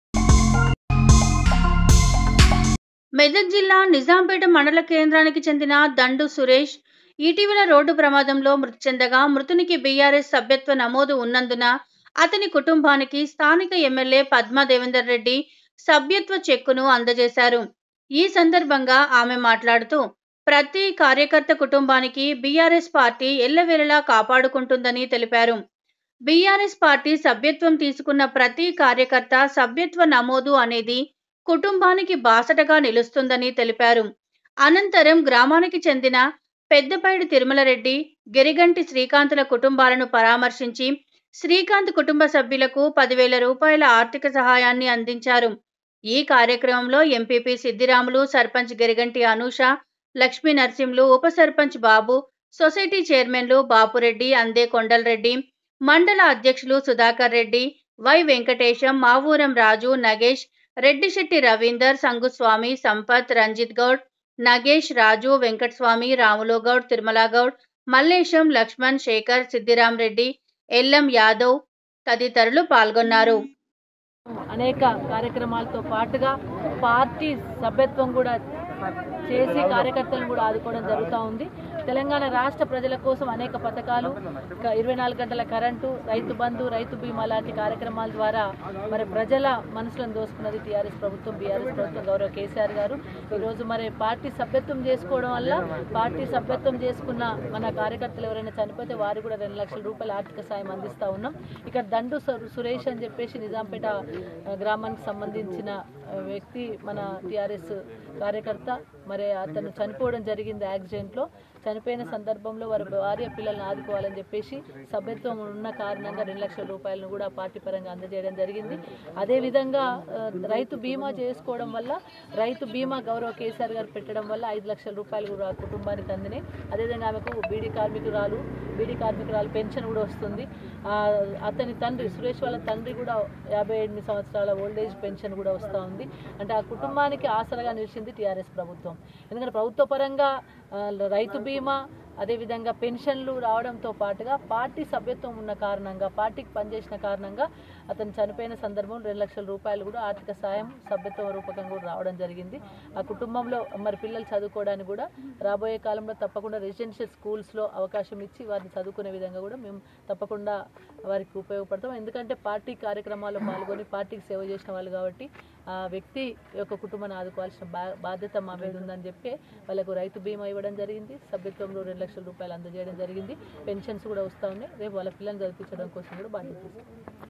నిజాంపేట మండలనికి చెందిన దండు సురేష్‌ మృతి చెందగా అతనికుటుంబానికి స్థానిక సభ్యత్వ చెక్కును అందజేశారు
నిజాంపేట మండలనికి చెందిన దండు సురేష్‌ కి బిఆర్‌ఎస్‌ సభ్యత్వ నమోదు ఉన్నందున అతని కుటుంబానికి  స్థానిక ఎమ్మేల్యే పద్మాదేవెందర్‌ రెడ్డి సభ్యత్వ చెక్కును అందజేశారు